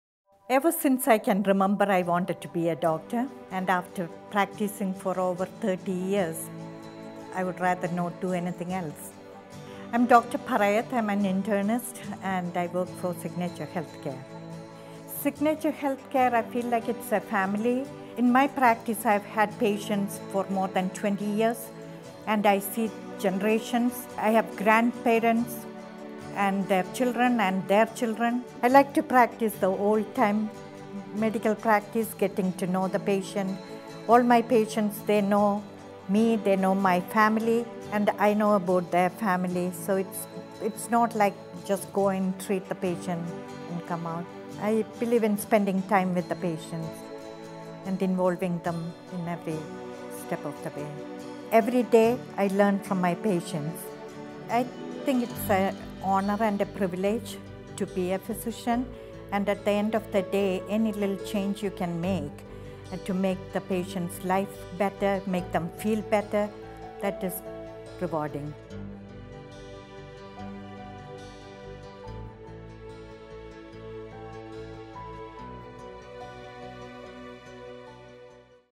Meet Dr. Padmaja Parayath - A Signature Physician
This video is part of a series of physician videos Jennings created for Signature Healthcare and Signature Medical Group. Jennings is a healthcare marketing and audience engagement firm.